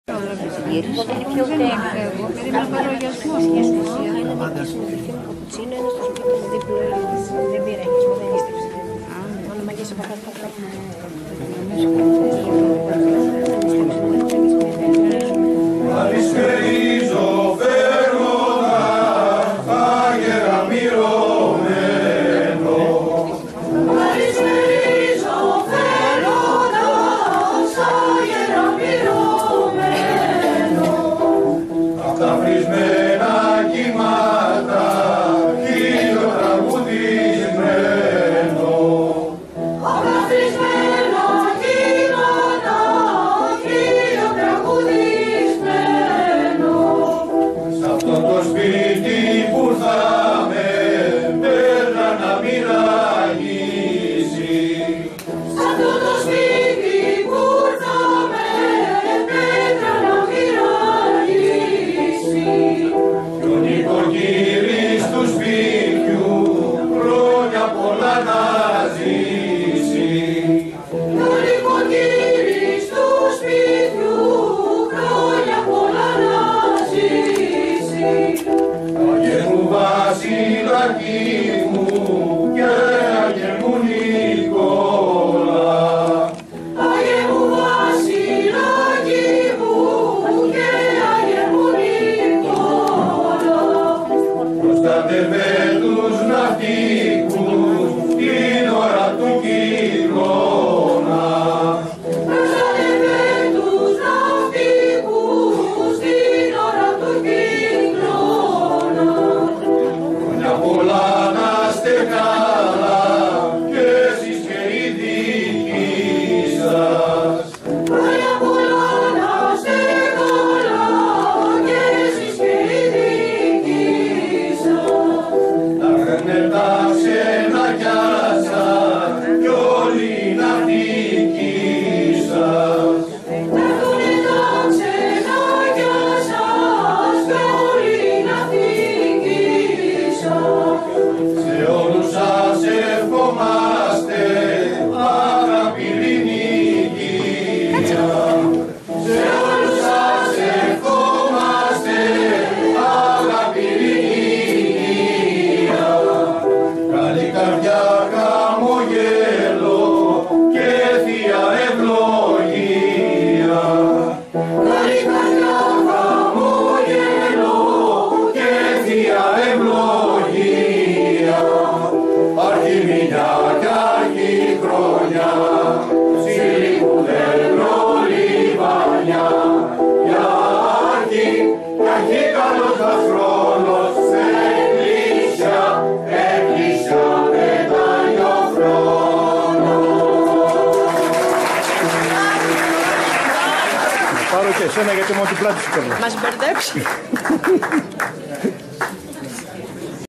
Πρωτοχρονιάτικα Παινέματα Χίου
Η ΧΩΡΟΔΙΑ ΤΟΥ ΠΡΟΟΔΕΥΤΙΚΟΥ  ΣΥΛΛΟΓΟΥ  ΑΡΕΤΣΟΥΣ & ΔΕΡΚΩΝ ΘΕΟΦΑΝΕΙΑ 6 ΙΑΝΟΥΑΡΙΟΥ 2016 ΣΤΗΝ  ΚΟΠΗ  ΠΡΩΤΟΧΡΟΝΙΑΤΙΚΗΣ ΠΙΤΑΣ ΤΟΥ ΣΥΛΛΟΓΟΥ ΣΤΟ ΠΟΛΙΤΙΣΤΙΚΟ ΚΕΝΤΡΟ ΠΡΟΠΟΝΤΙΔΑ   (ΧΟΡΑΡΧΗΣ  ΣΟΦΙΑ  ΝΑΤΣΙΟΥ)